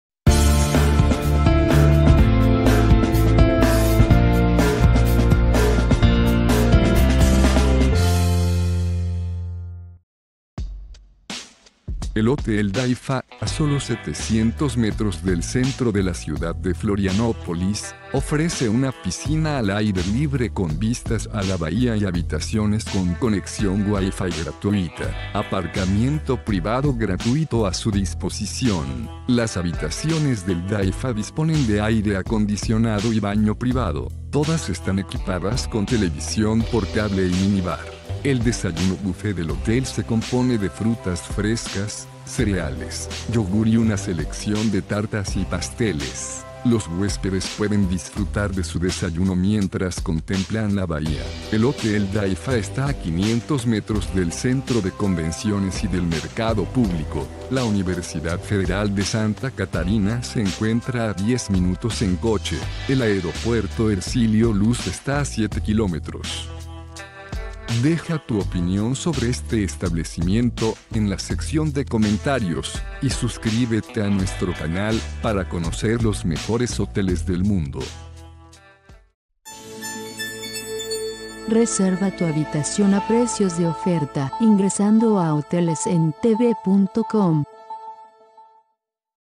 Hotel Daifa, Florianópolis
Hotel Daifa es un adorable Hotel de Florianópolis con servicio de habitaciones, recepción 24 horas y prensa . Este alojamiento que ingresó al sistema en el año [año de ingreso] ofrece 20 habitaciones con minibar, ducha y caja fuerte.